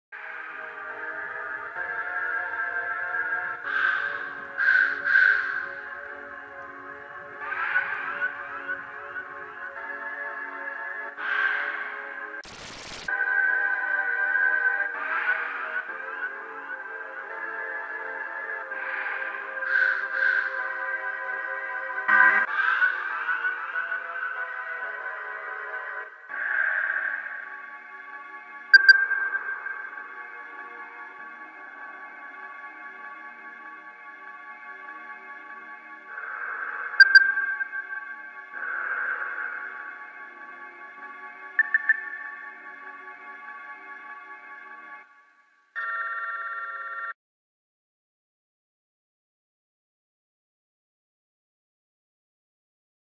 funny 80s platypus commercial
its a weird commercial that i found on a VHS tape from that platypus that looks like perry